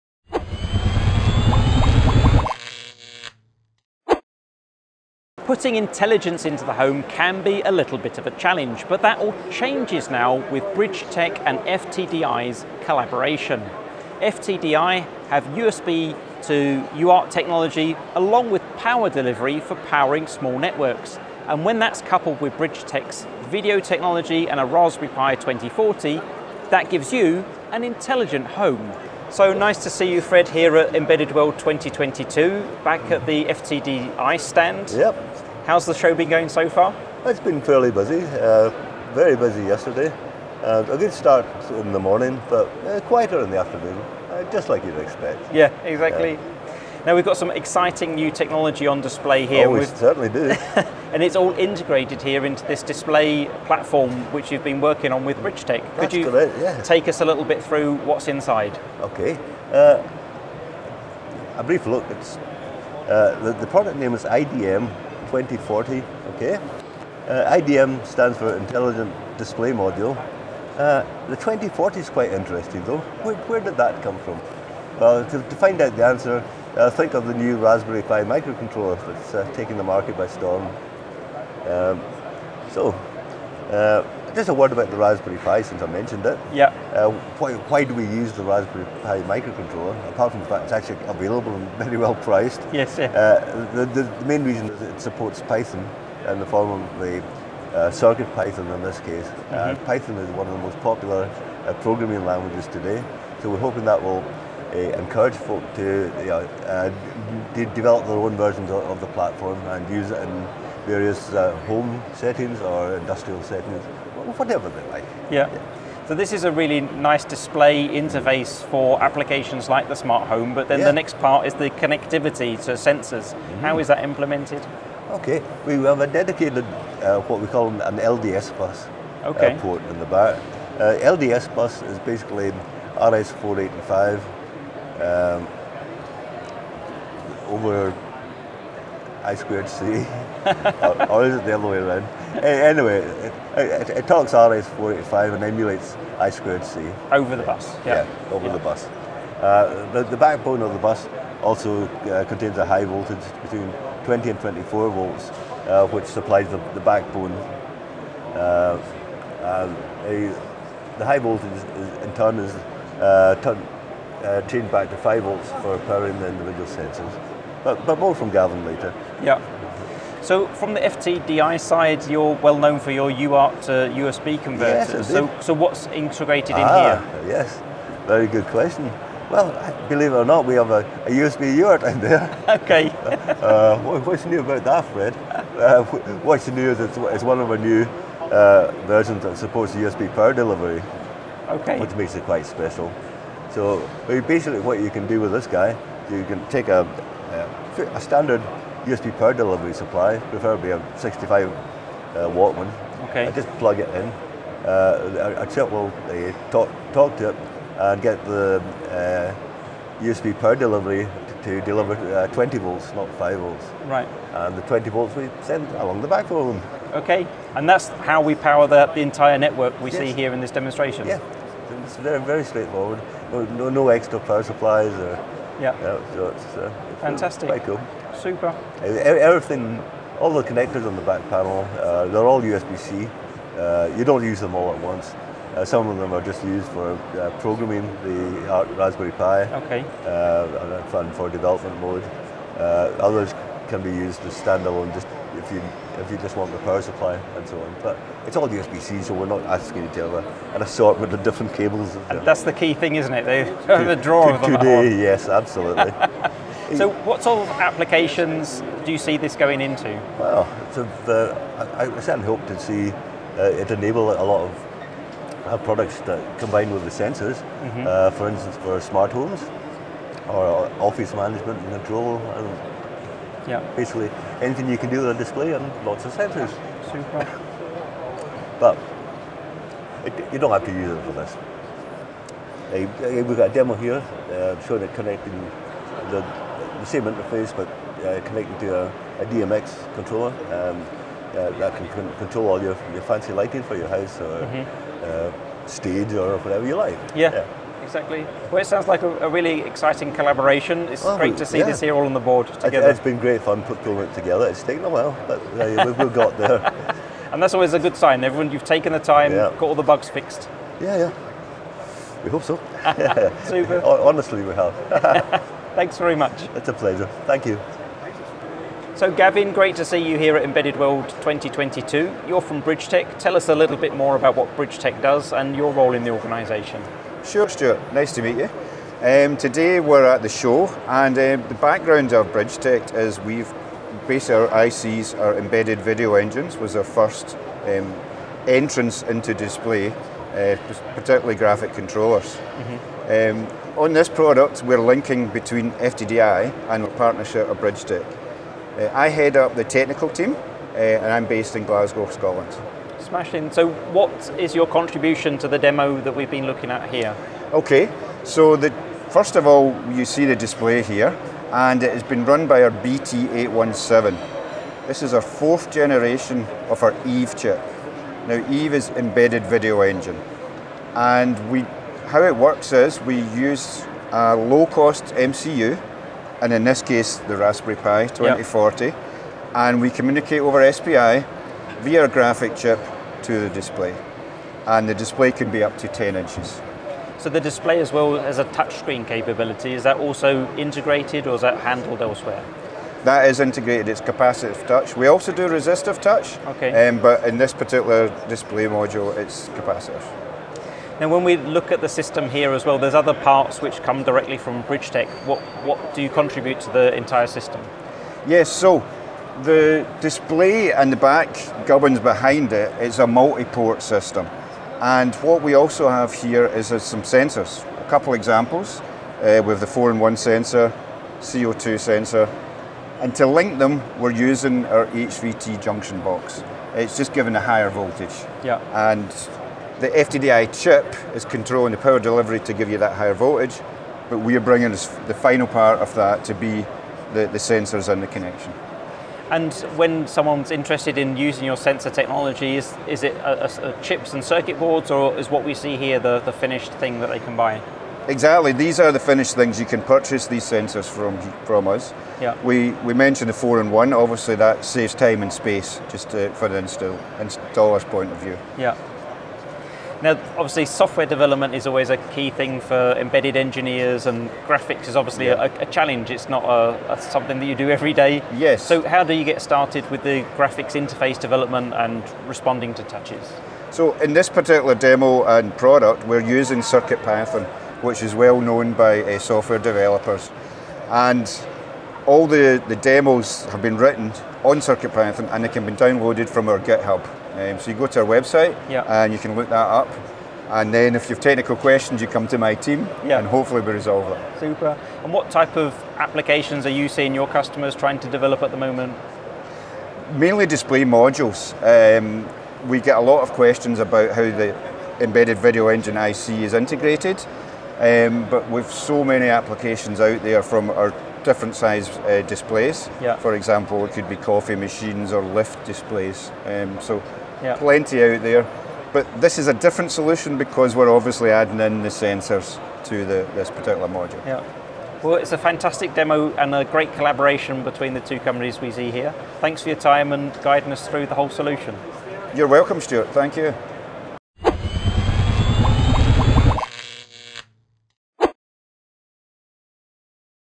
Elektor Talks to FTDI and Bridgetek at Embedded World 2022
FTDI and Bridgetek at Embedded World 2022: IDM2040-7A, FT232HP, and EVE BT817 with Fred Dart and Gavin Moore.

Bringing intelligence into the home and buildings can be challenging, but no longer! This collaboration between FTDI and Bridgetek brings a touch-screen interface together with wired connectivity powered by USB-PD - great for the smart home and building automation. Discover more about the FTDI Uart-to-USB-PD bridge

Visit the Elektor Mag website to learn more about these companies and topics such as building automation and home automation